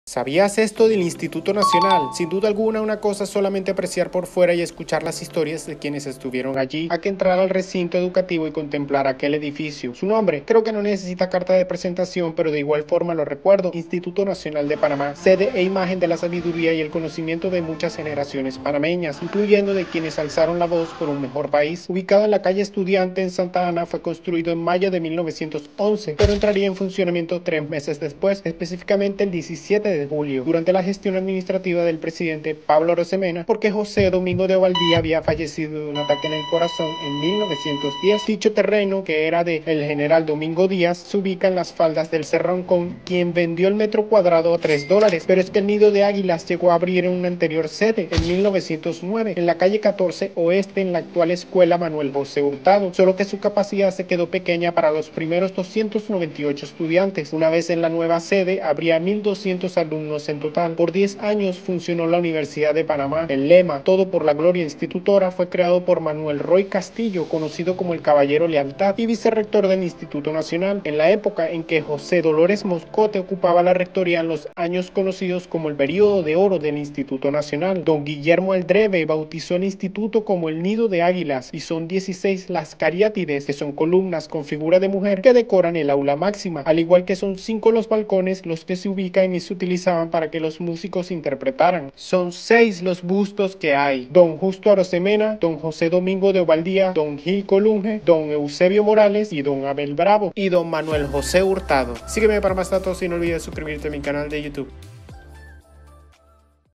Breve historia del "Nido de Águilas" (Instituto Nacional de Panamá)
Sin duda alguna, una cosa es solamente apreciar por fuera y escuchar las historias de quienes estudiaron allí, a que entrar al recinto educativo y contemplar aquel edificio. ¿Su nombre? Creo que no necesita carta de presentación, pero de igual forma lo recuerdo: Instituto Nacional de Panamá, sede e imagen de la sabiduría y el conocimiento de muchas generaciones panameñas, incluyendo de quienes alzaron la voz por un mejor país.

Undoubtedly, it is one thing to just appreciate the outside and listen to the stories of those who studied there, than to enter the educational campus and contemplate that building. Your name? I don't think you need a cover letter, but I still remember it: National Institute of Panama, headquarters and image of the wisdom and knowledge of many Panamanian generations, including those who raised their voices for a better country.

Videos & fotos & edición: @matos0893 (Andrés Matos) & Internet.